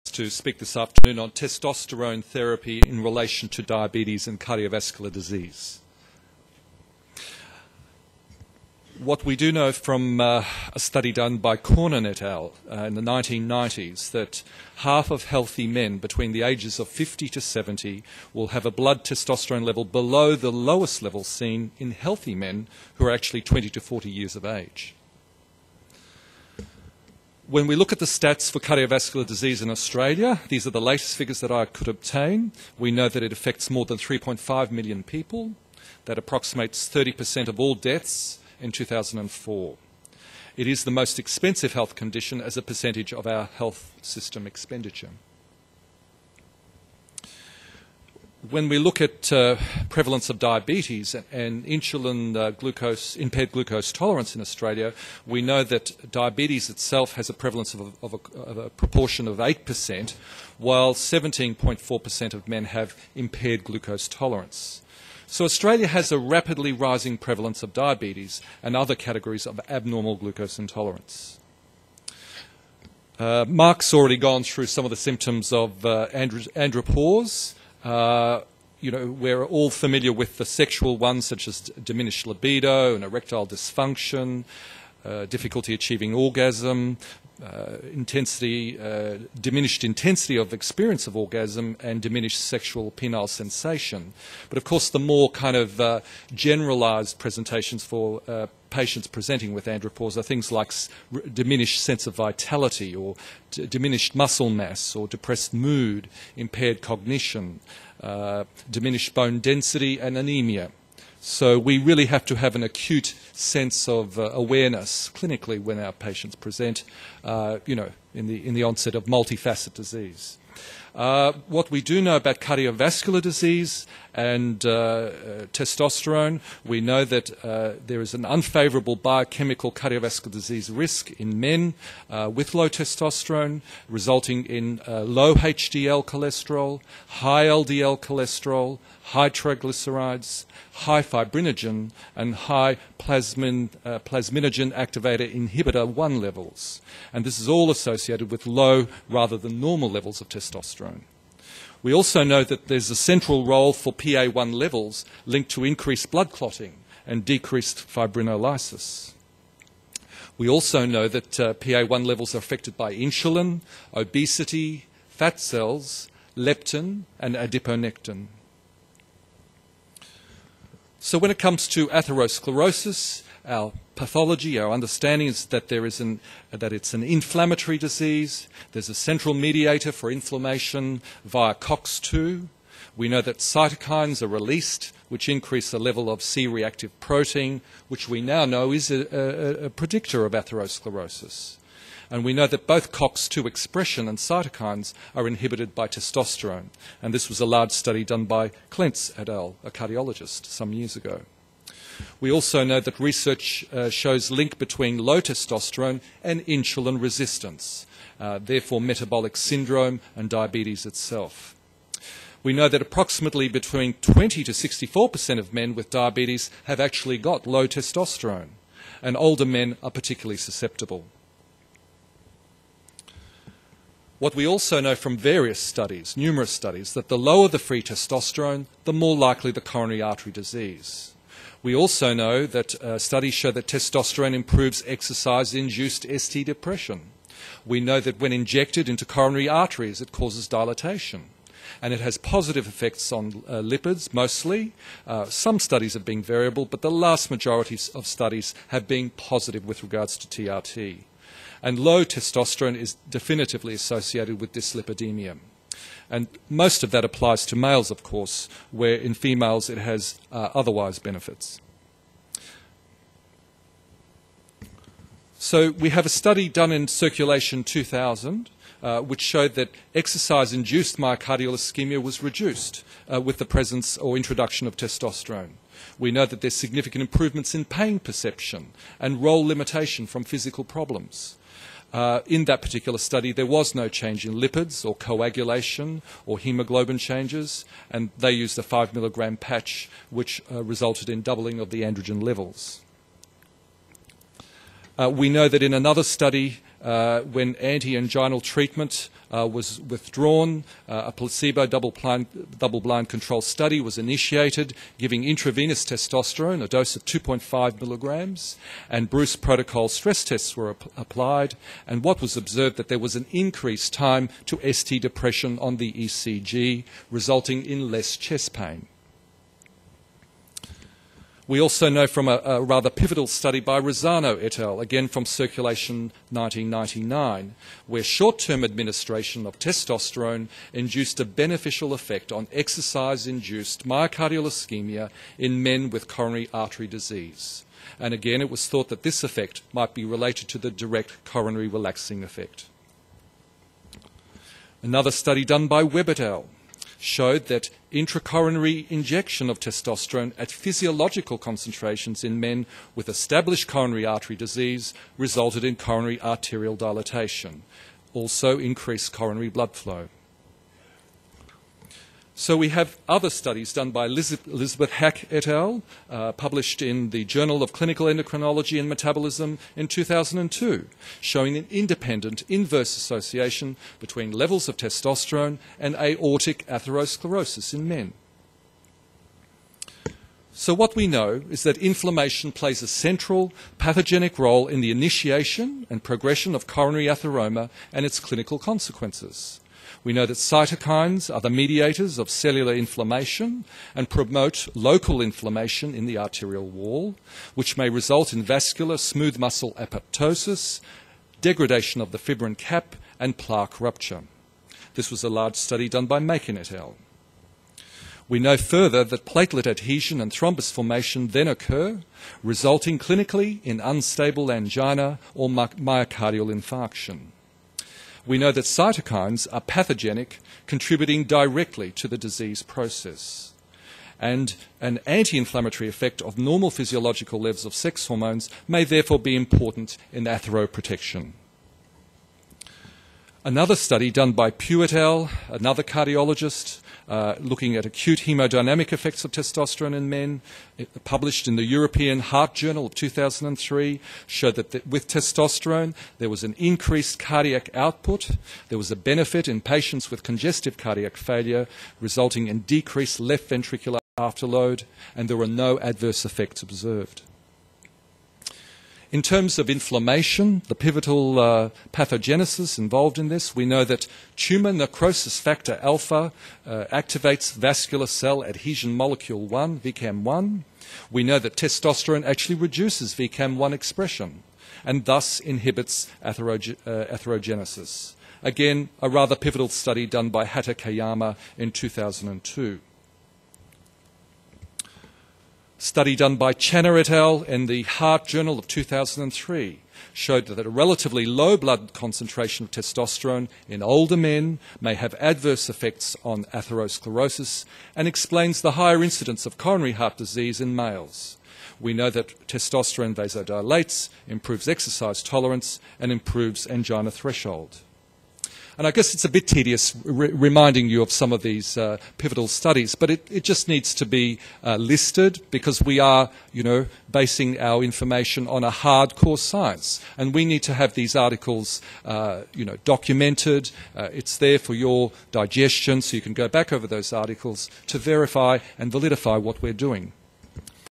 Testosterone in Treating Diabetes and Heart Disease - Dr. Denis Rebic
This is a preview of Testosterone in Treating Diabetes and Heart Disease by Dr. Denis Rebic at the 2008 A5M Australia Conference. to purchase this lecture and many others on DiGiVision, video and slides synced. for the conference sponsor.
Item A5M-081W26